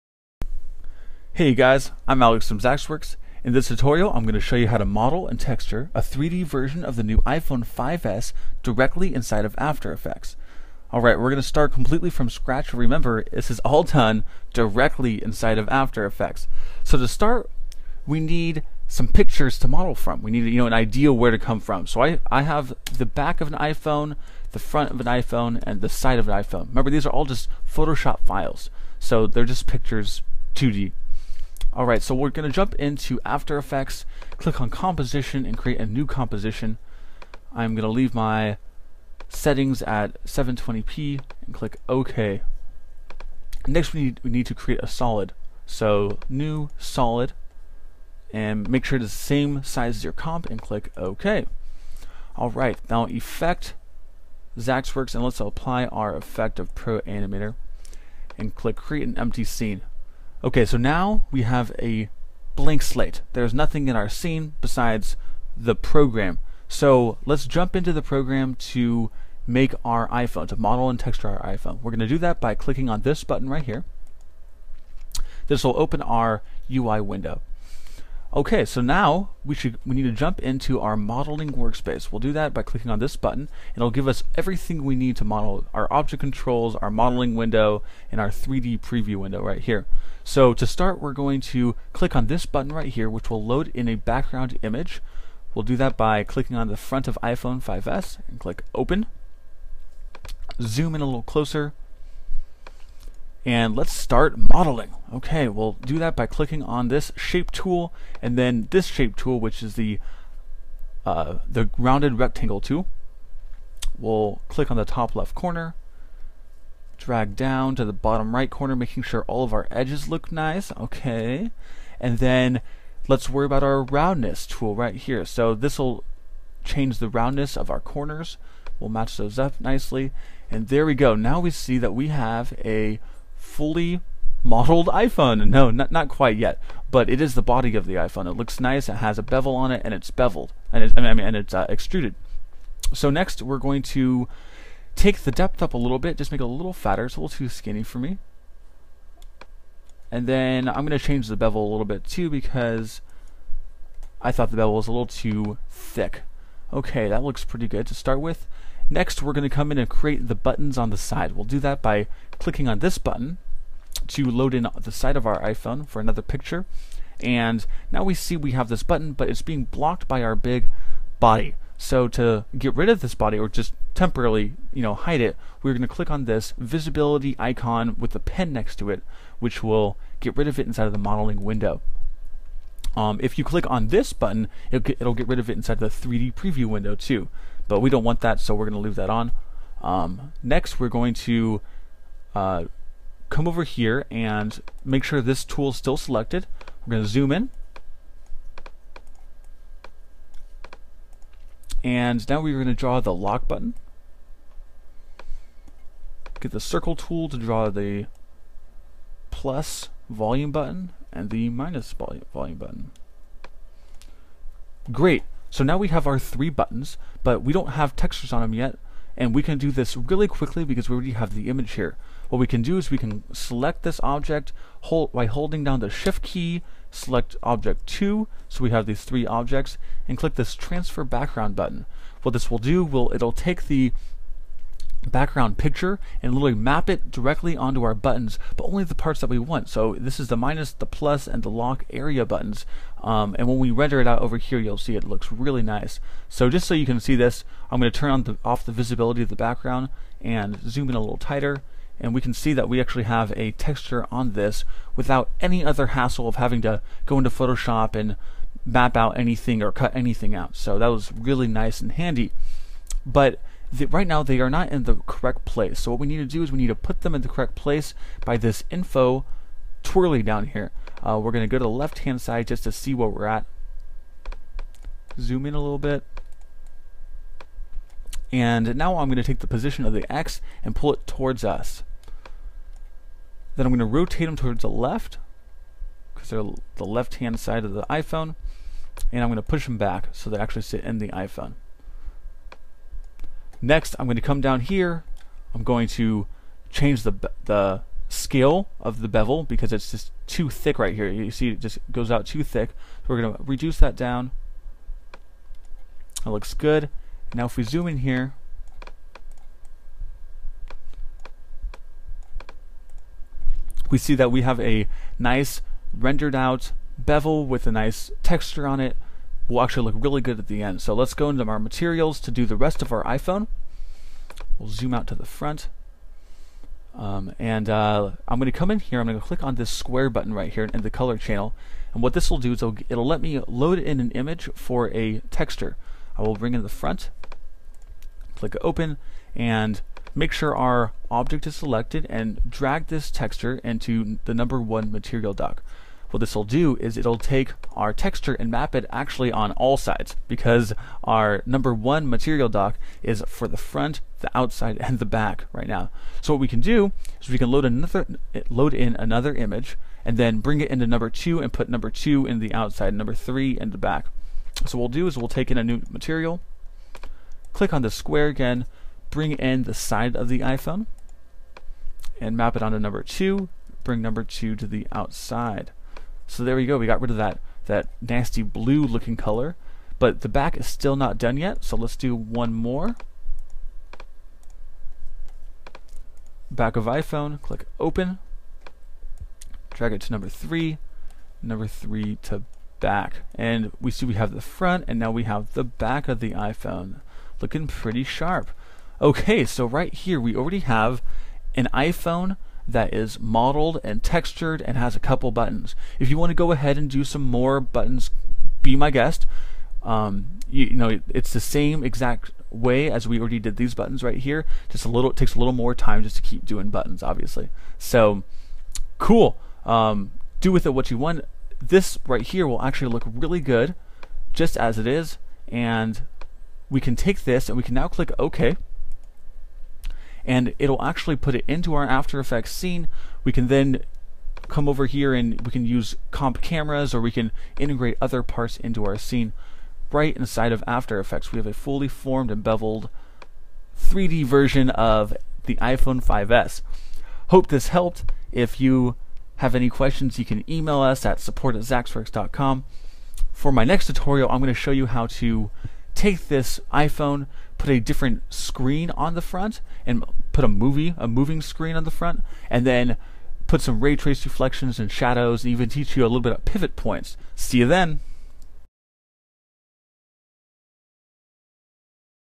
Tutorial 63 - Building a 3D iPhone 5S in After Effects in under 8 minutes - Part 1
In this tutorial, we'll show you how easy it is to create an iPhone in After Effects using our 3D ProAnimator Plug-in.  This tutorial only takes 8 minutes, with explanation, and leaves you with a fully modeled and textured 3D iPhone 5S.